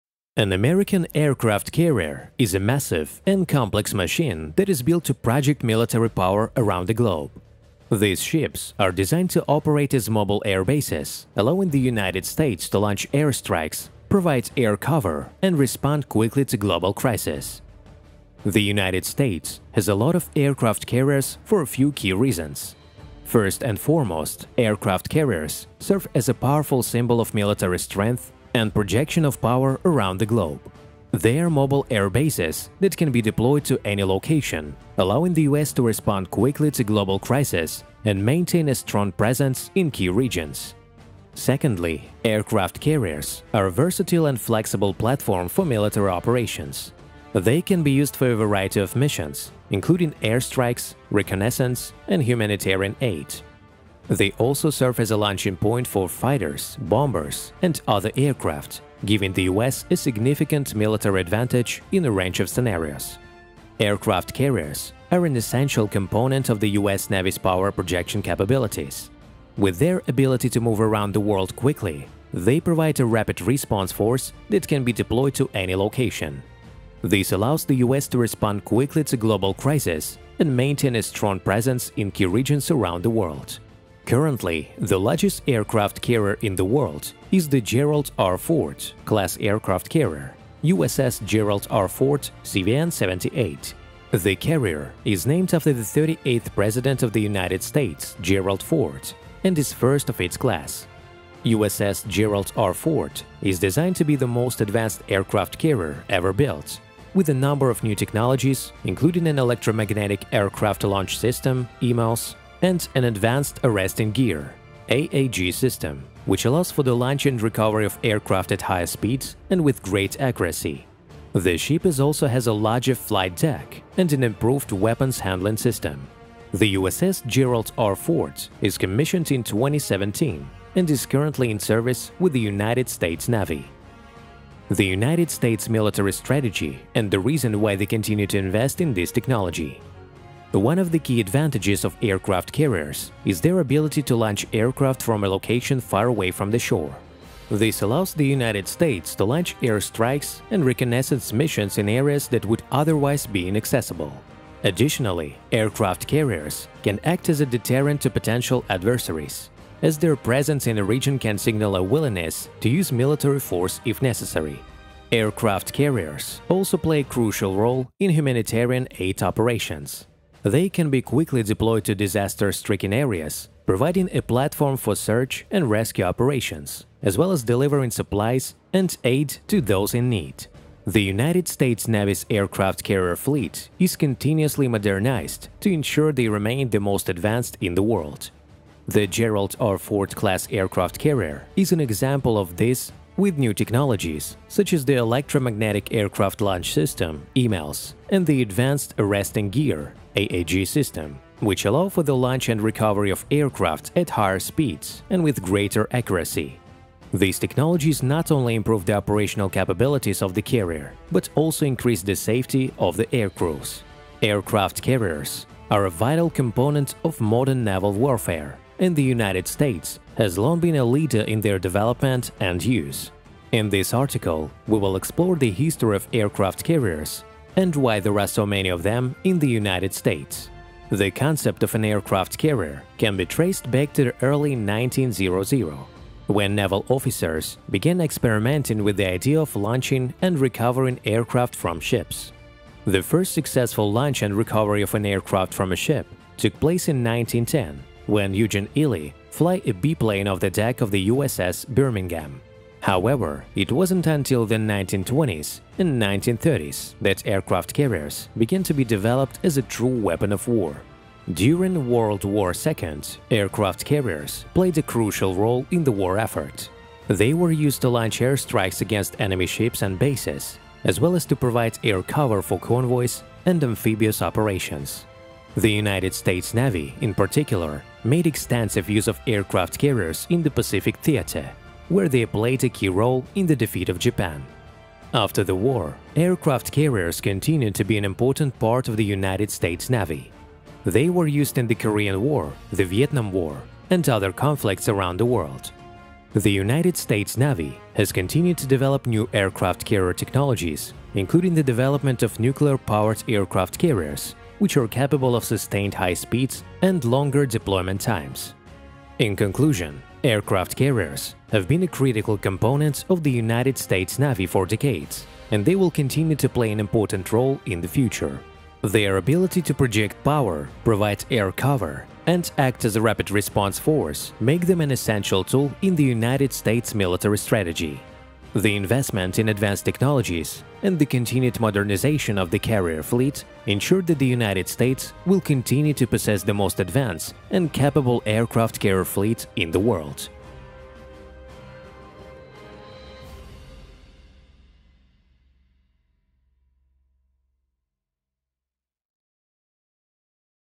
Why is the whole WORLD afraid of them? What are US Navy aircraft carriers capable of?
Discover the history of these massive ships, from their origins in World War II to their current role in America's military strategy. See how these floating cities work, from the flight deck to the engine room, and learn about the advanced technology that makes them some of the most powerful ships in the world. Explore the role of aircraft carriers in modern warfare and how they are used to project American military power around the globe. Get an inside look at the daily operations of these ships and the men and women who operate them. This video will give you an unparalleled understanding of America's aircraft carrier fleet and its importance in the world today.